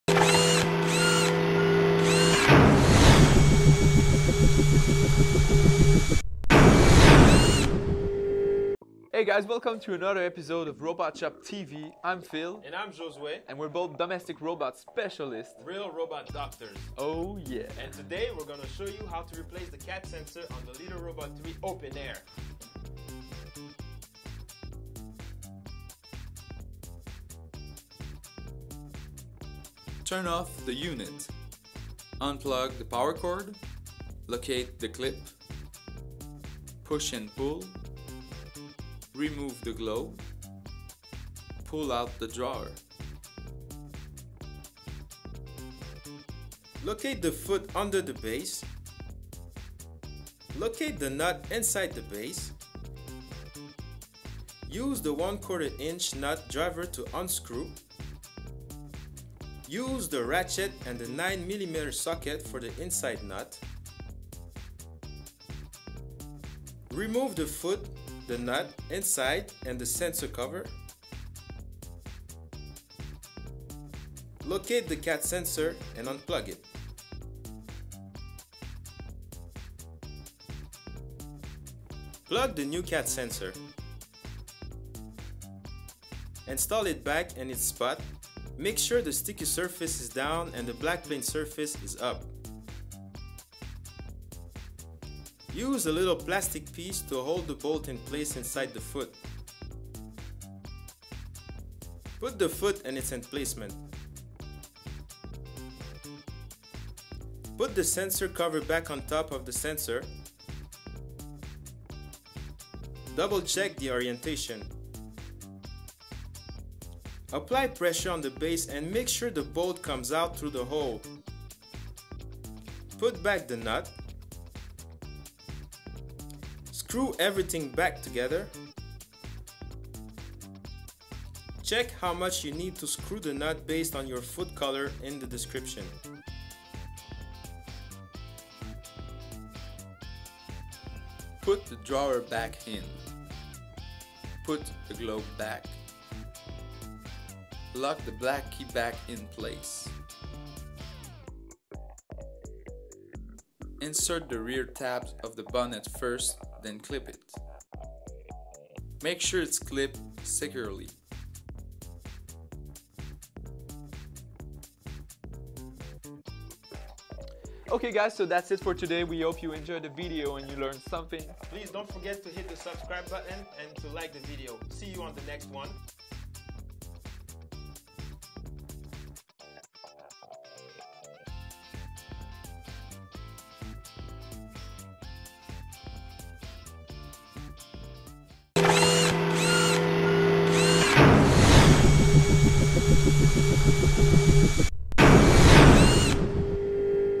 How To Repair The Cat Sensor In The Litter-Robot 3 Open Air
In this video, our Domestic Robots Specialists Phil and Josué teach you how you can easily replace the cat sensor in your robotic litter Litter-Robot 3 Open Air.

If your foot is white (and the bolt is 1 7/8'' cm) : you need to screw for the bolt to be about ~1/4" (6.35mm) from the bolt.
If your foot is black (and the bolt is 1 5/8'' cm) : you need to screw for the bolt to just be a mm from the bolt.

It's essential that you respect these directions to not crush the cat sensor.